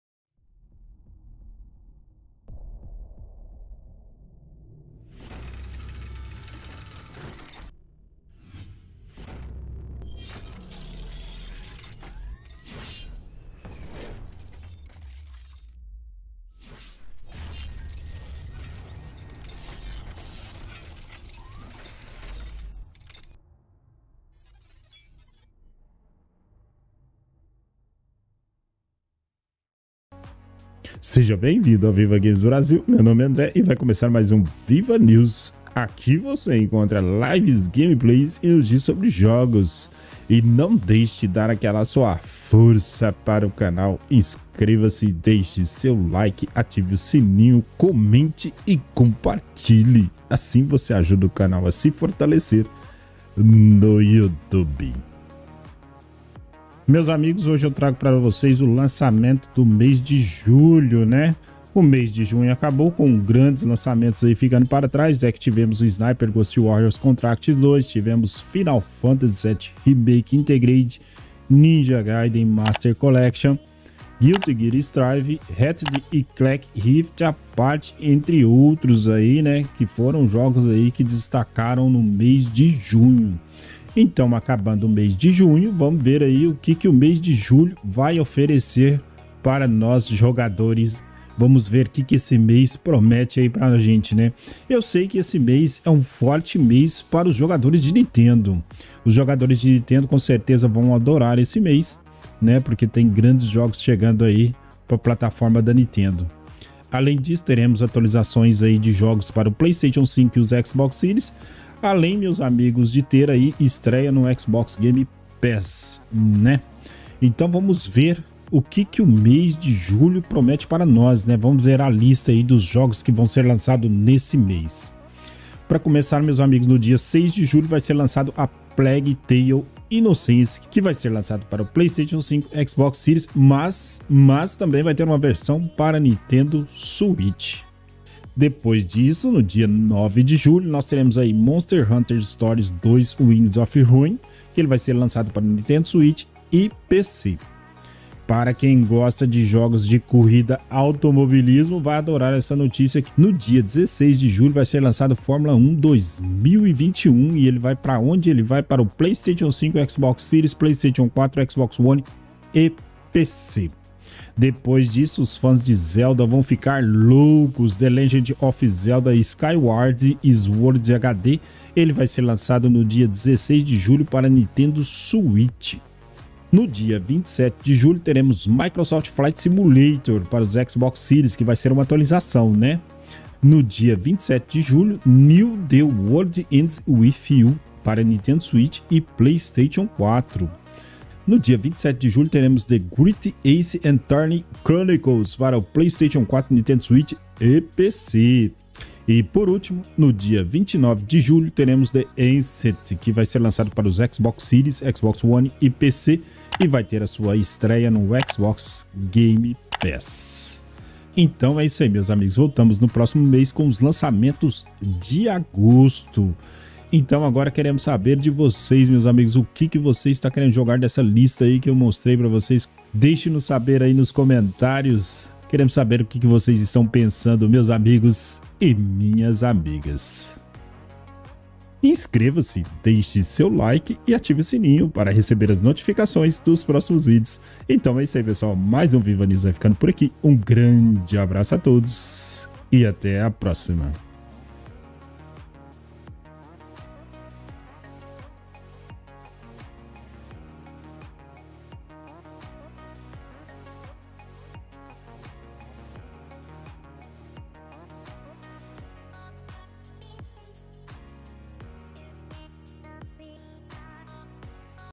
Lançamentos de games / jogos do Mês - Julho 2021 (PS5, PS4, Xbox Series, Xbox one, Switch e PC )
Lançamentos de games / jogos do Mês Julho 2021 para PS5, PS4, Xbox Series, Xbox one, Switch e PC

Se você gostou do vídeo, lembre-se de deixar um curtir e comentar, agradeço muito